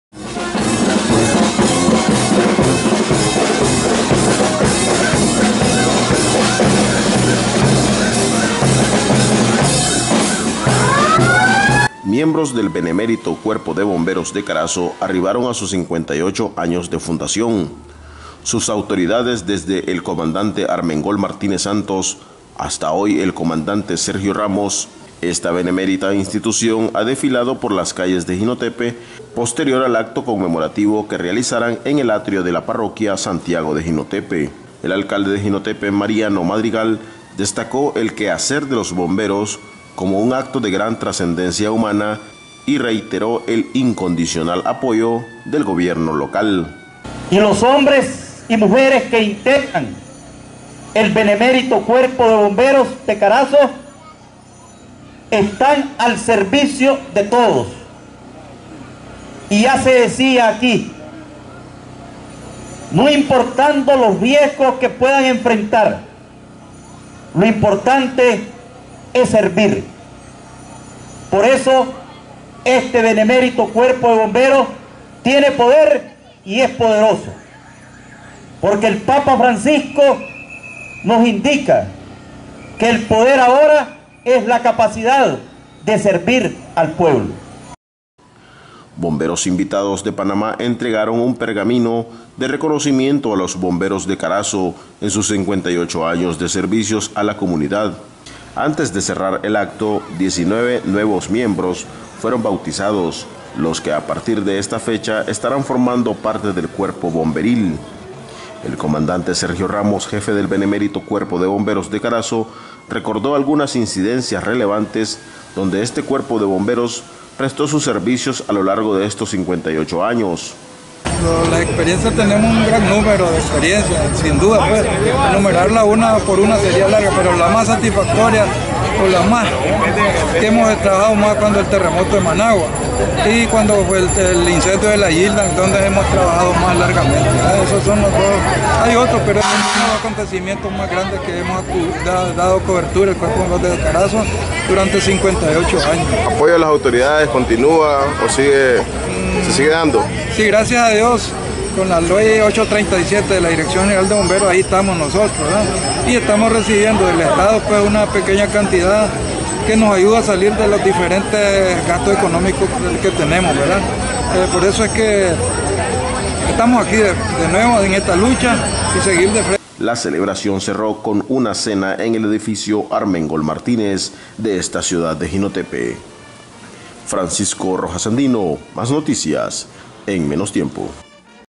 BENEMERITO CUERPO DE BOMBEROS DE CARAZO CUMPLIERON 58 AÑOS
BOMBERO DE CARAZO CUMPLEN 58 AÑOS